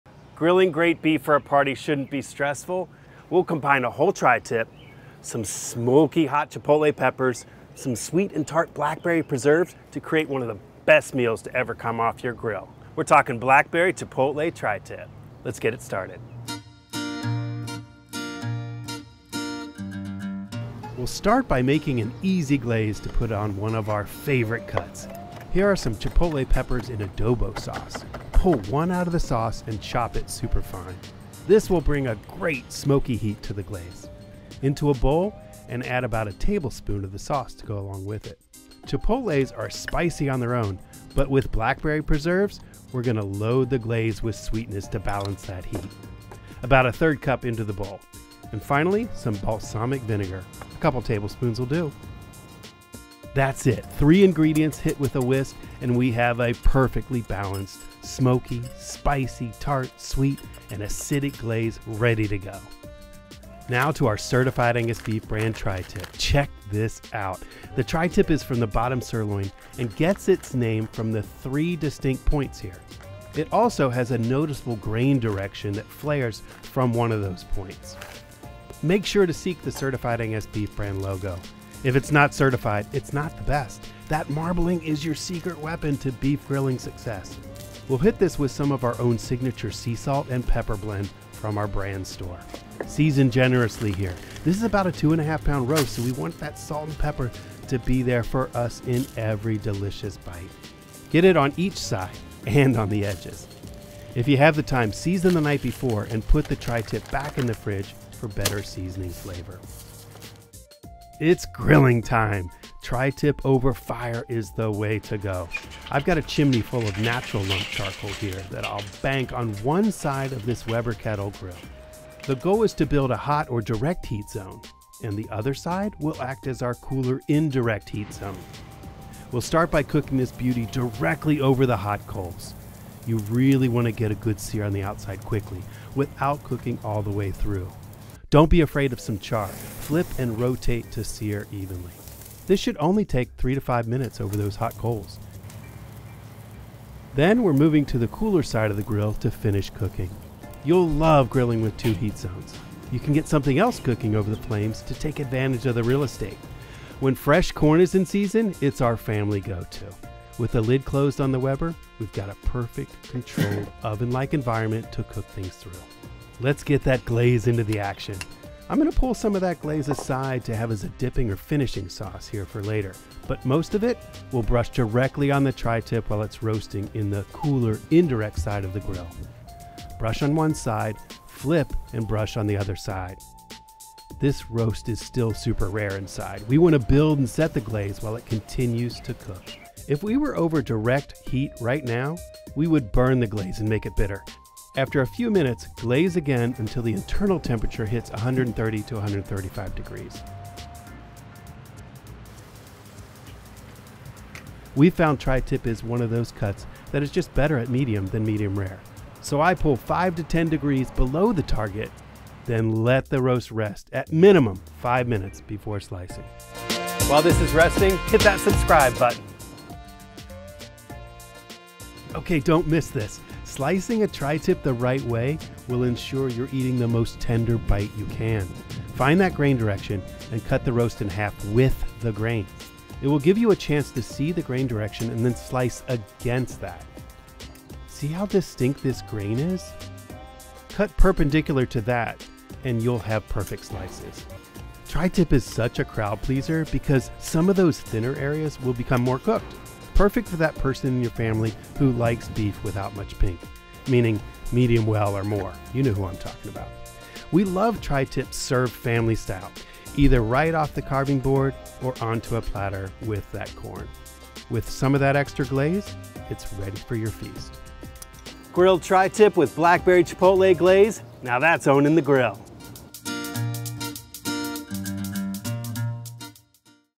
Blackberry Chipotle Glazed Tri-Tip Recipe
A little sugar. A little spice. Everything’s nice with this Blackberry Glazed Tri-Tip. The tri-tip is a juicy, tender beef cut when grilled to a perfect medium degree of doneness and pairs well with your favorite summertime sides. Watch to see how you can build greater flavor on the grill with a homemade glaze, the right amount of heat and a proper slicing technique.

INGREDIENTS:
1 (2-2 1/2 pound) Certified Angus Beef ® tri-tip roast, trimmed of excess fat
2 teaspoons kosher salt
1 teaspoon black pepper
1/3 cup blackberry preserves
2 tablespoons balsamic vinegar
1 canned chipotle pepper, minced
1 tablespoon adobo from canned chipotle

Salt & Pepper Cellar (Himalayan Flake Salt + 5 Peppercorn Mélange)